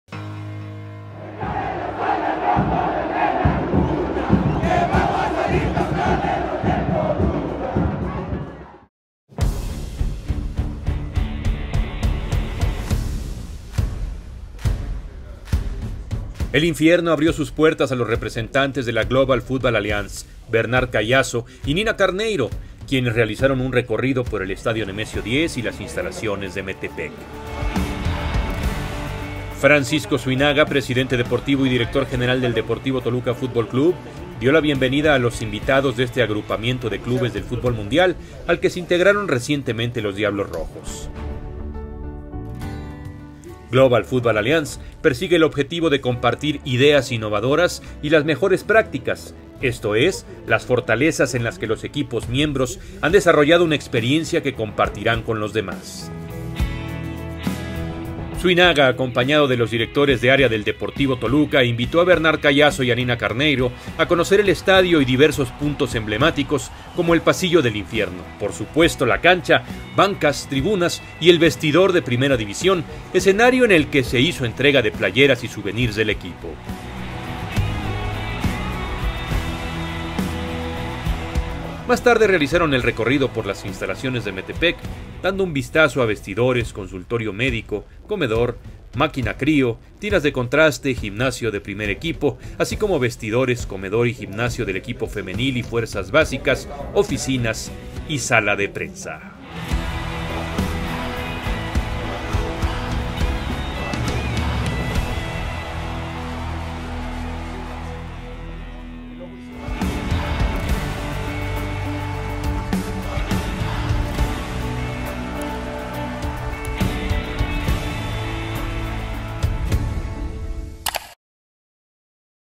La Global Football Alliance visita nuestro Infierno
Nuestros diablos ya forman parte de este agrupamiento del futbol mundial.